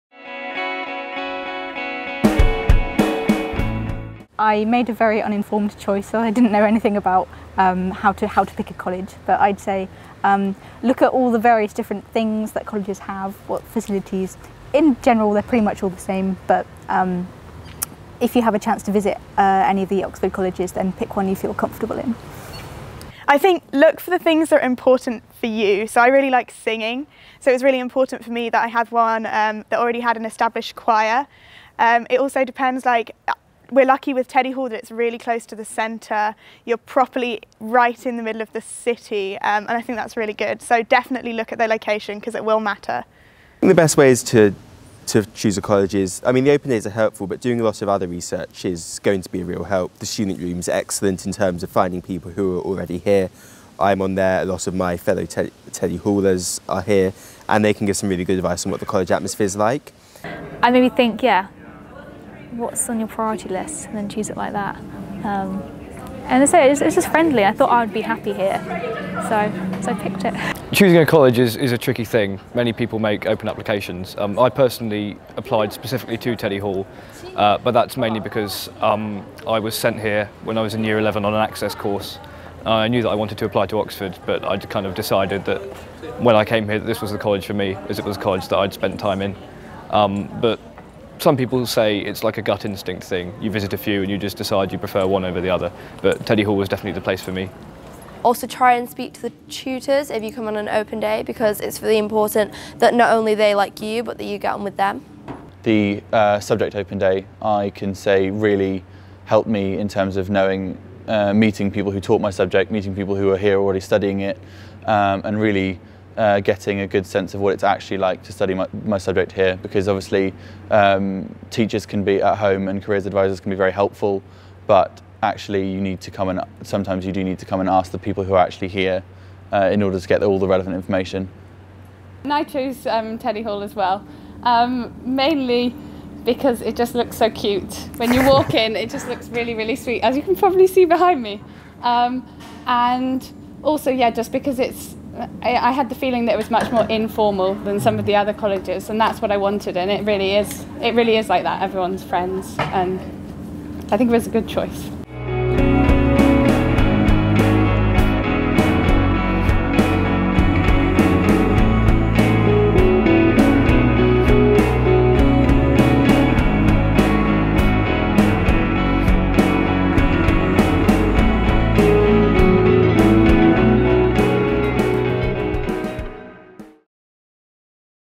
Choosing an Oxford college - advice from undergraduates at St Edmund Hall
Tips from students at St Edmund Hall (Teddy Hall) on how to choose a college when applying to the University of Oxford